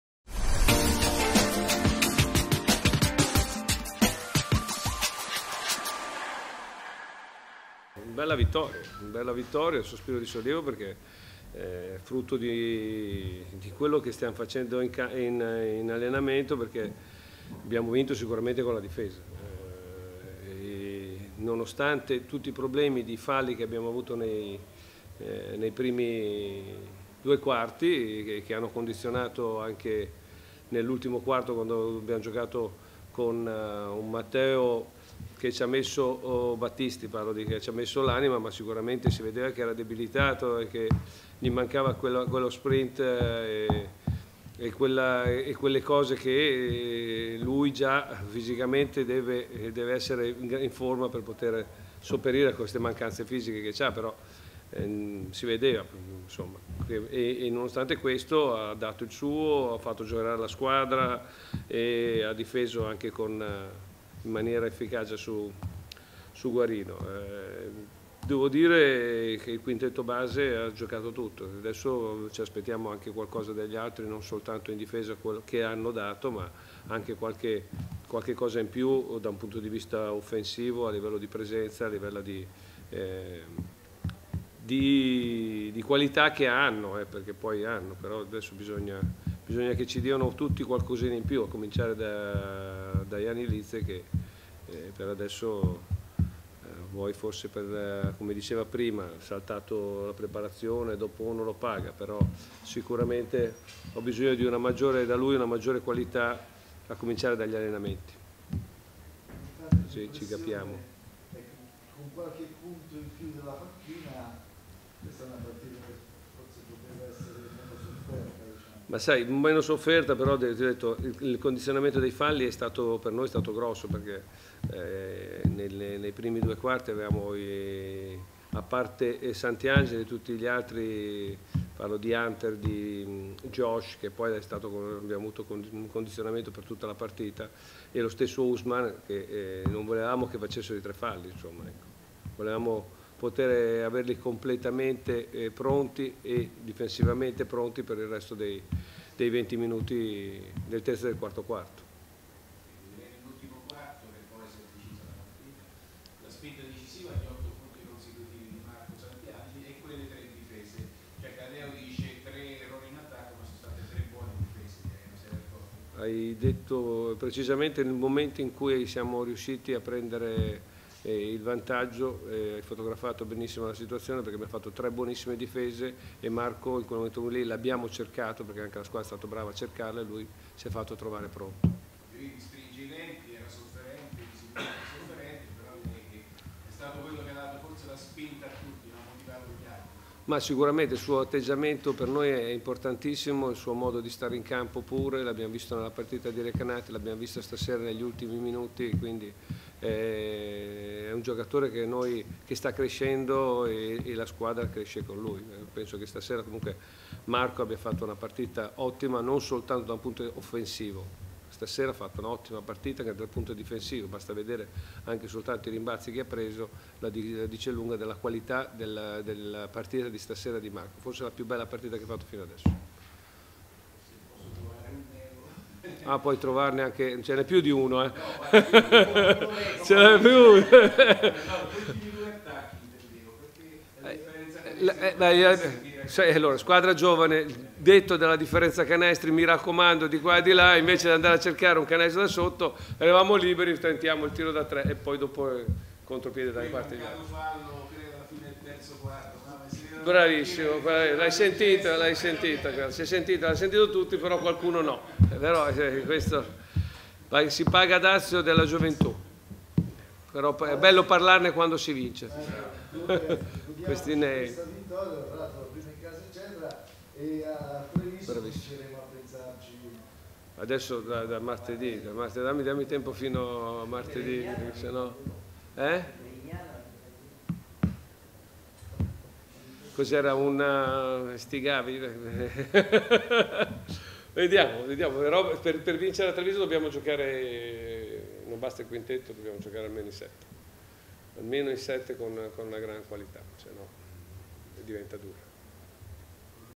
Aurora Basket vs Matera: 82-76 Parola a coach Maurizio Lasi
La felicità di Maurizio Lasi, premiato il lavoro settimanale.

Redazione Vallesina Tv